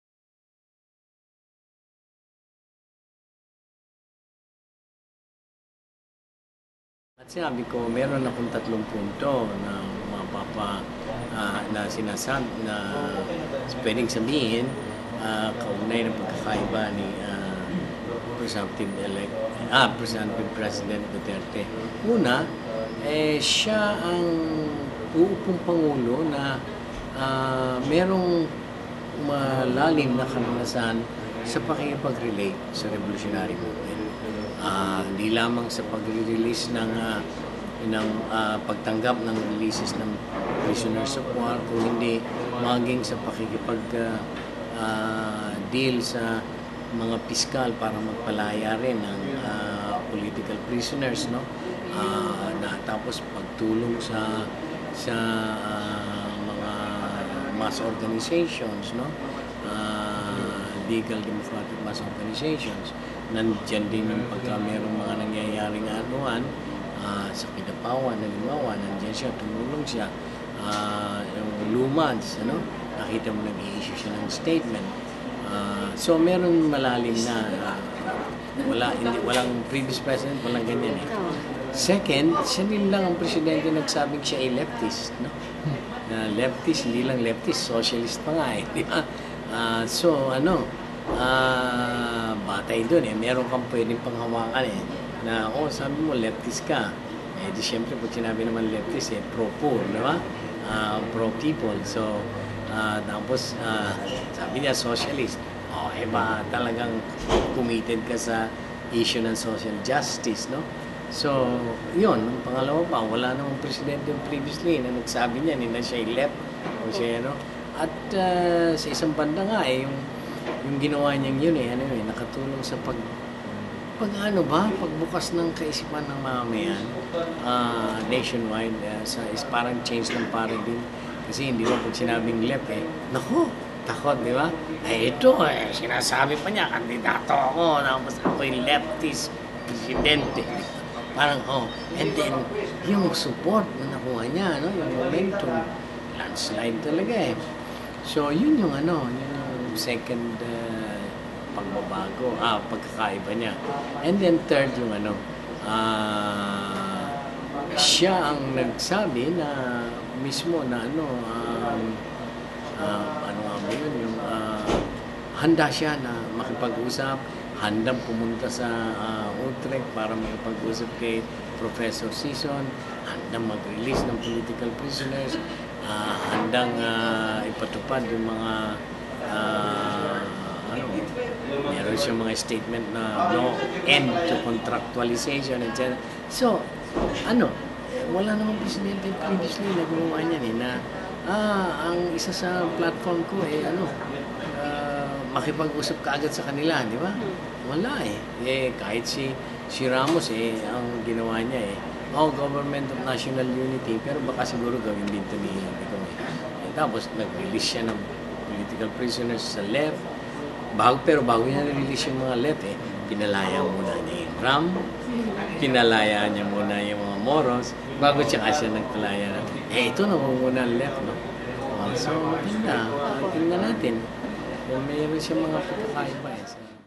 Duterte a distinct leader NDF
What makes Duterte distinct from past PH presidents? NDF gives us three reasons.